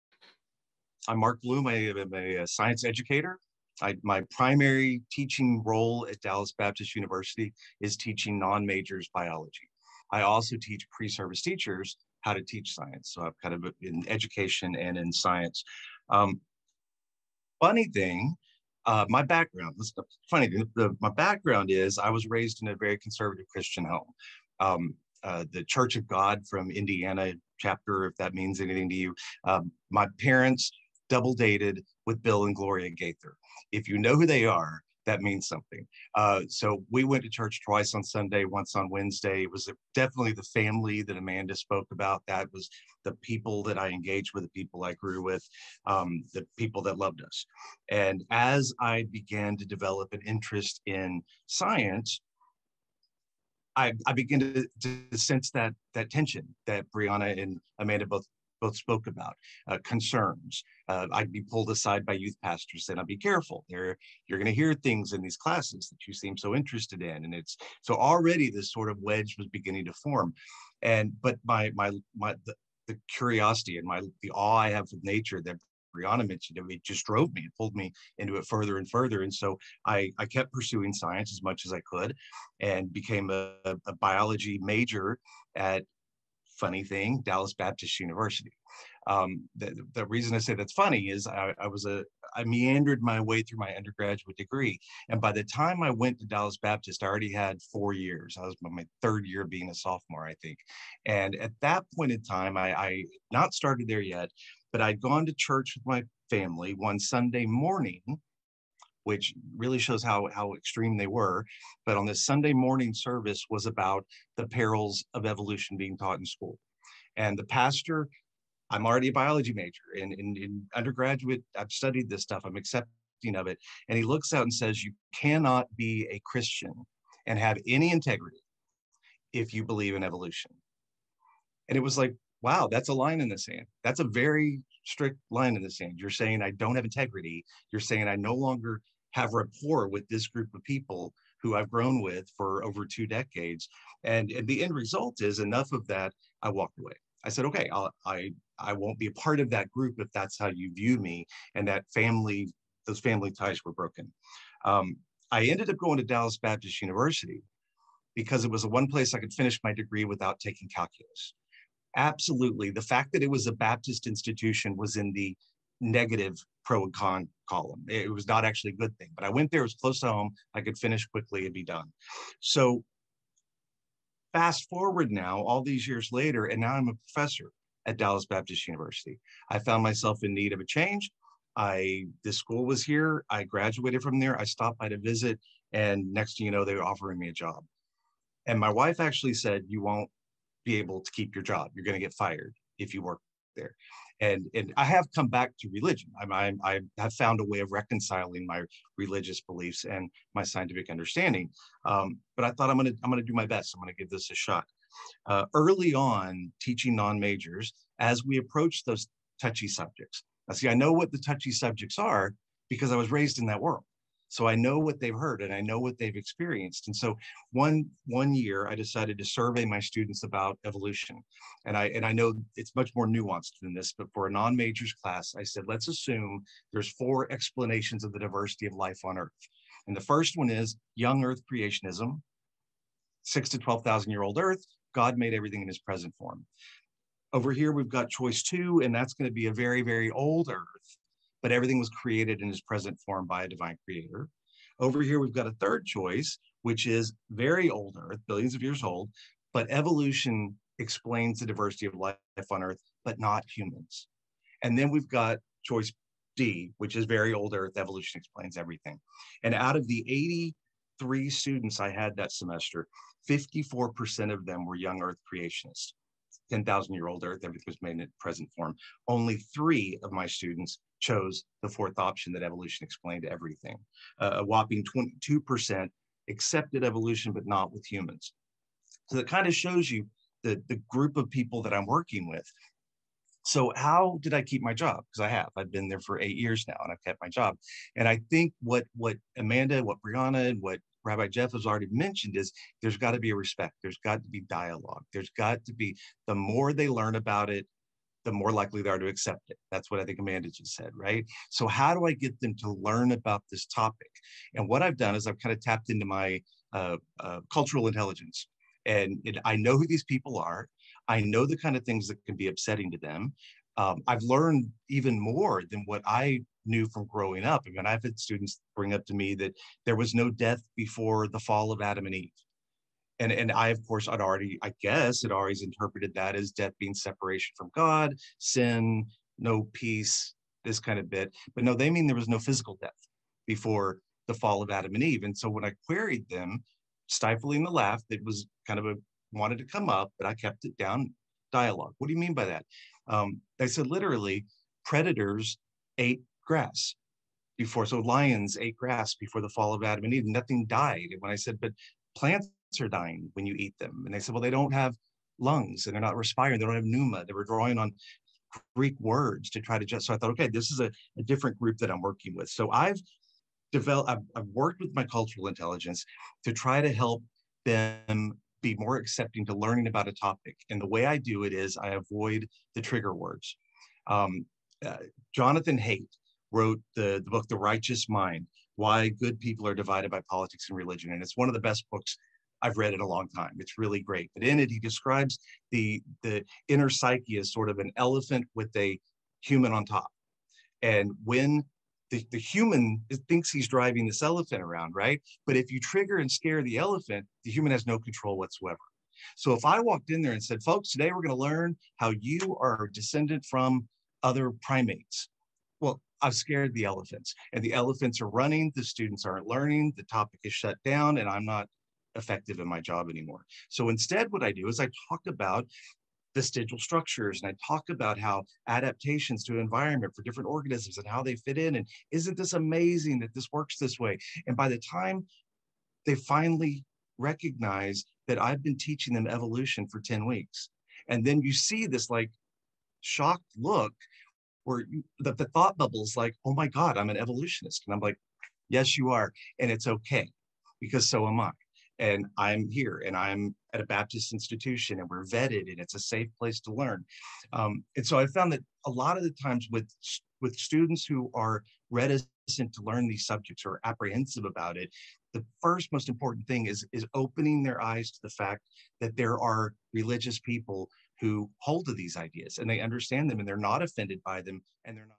Dr. Mark Bloom – Darwin Day at Shippensburg University
On February 11, 2021, Shippensburg University hosted its 13th annual Darwin Day event, in honor of Charles Darwin's birthday. This year, led by Briana Pobiner, a group of Sinai and Synapses Fellows "took over" the event, offering their personal stories on how they have experienced the conflict between science and religion in their lives, with a particular eye toward reaching new audiences and educating a potentially reluctant public. If you haven't met them already, these videos offer an excellent introduction to the Fellows and what inspired them to bridge the divide between the worlds of religion and science.